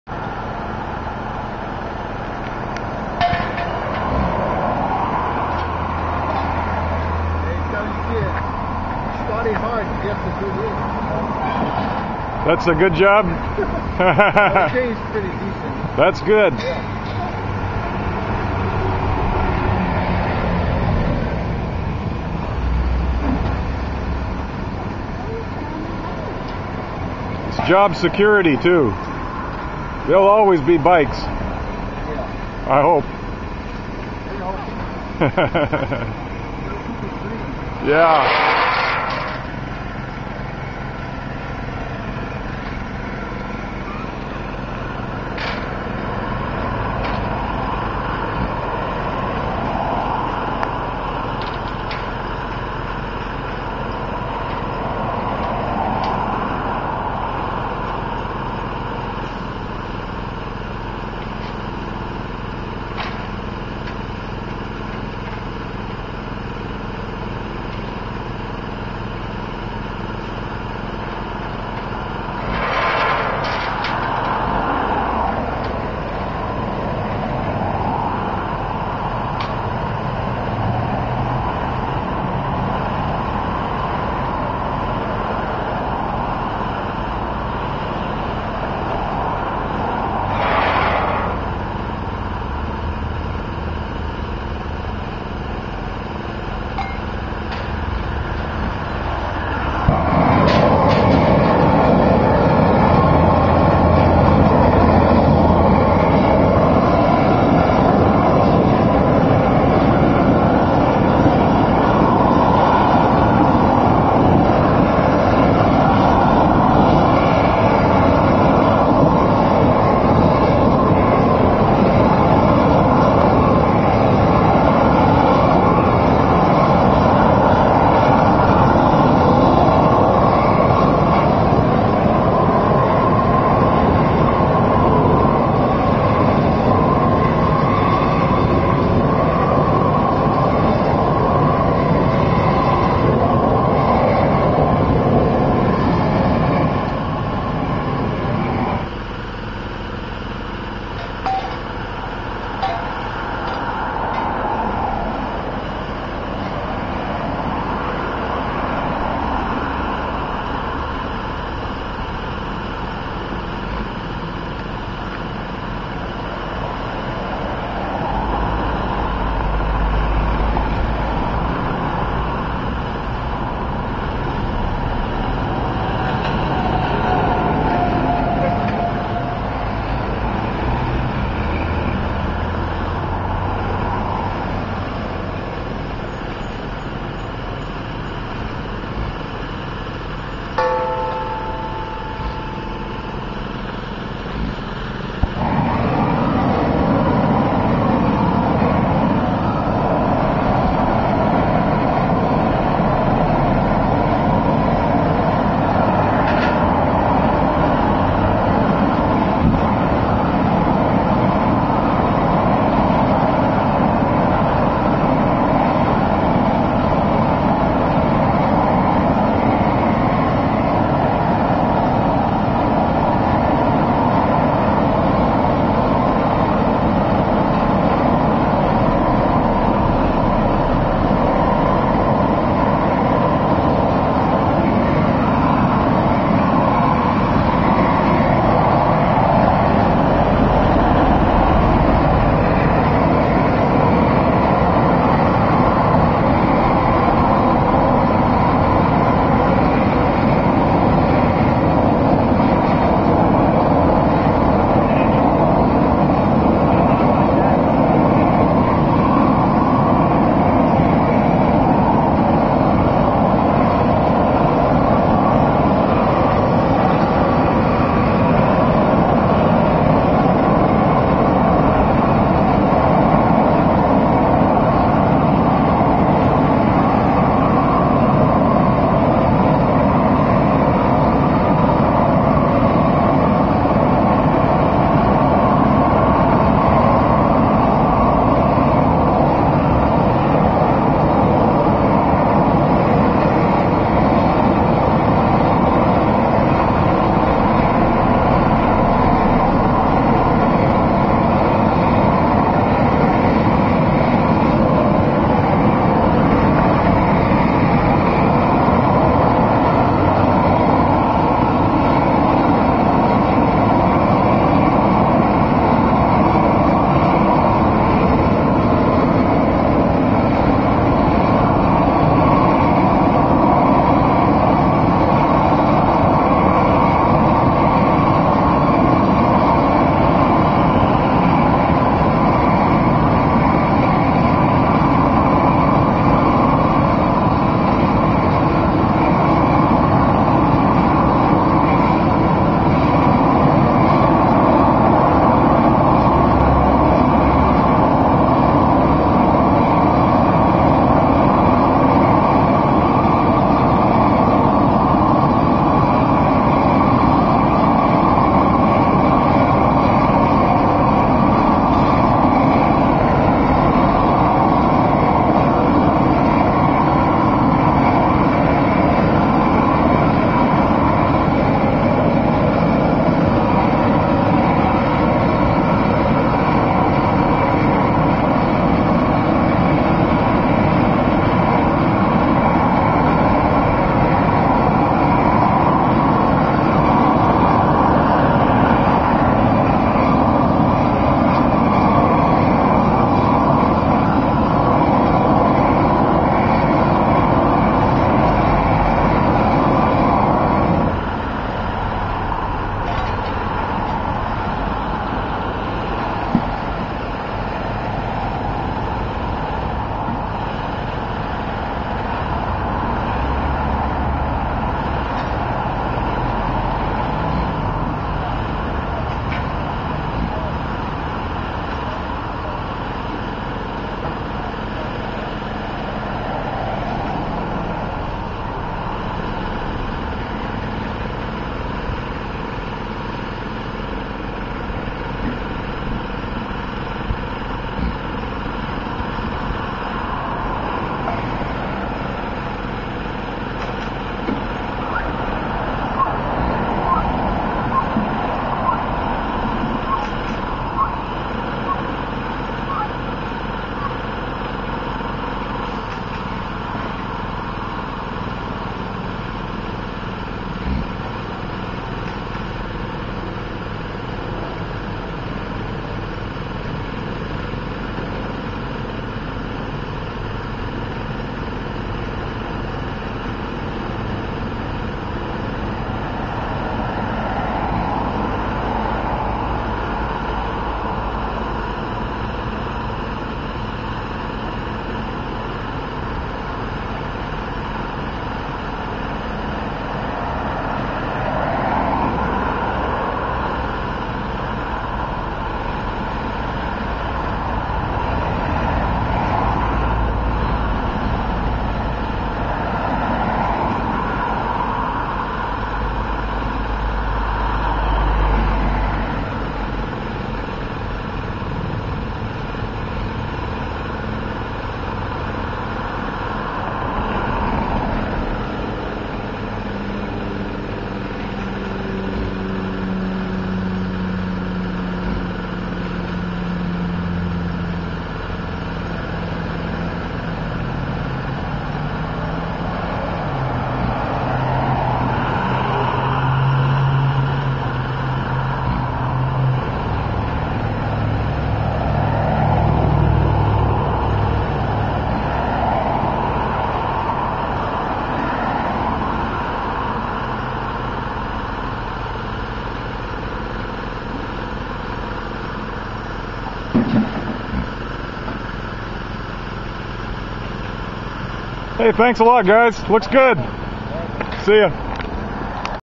Bike Lane Share Symbol Application
Workers in Seattle Applying a Bike Lane Share symbol on the street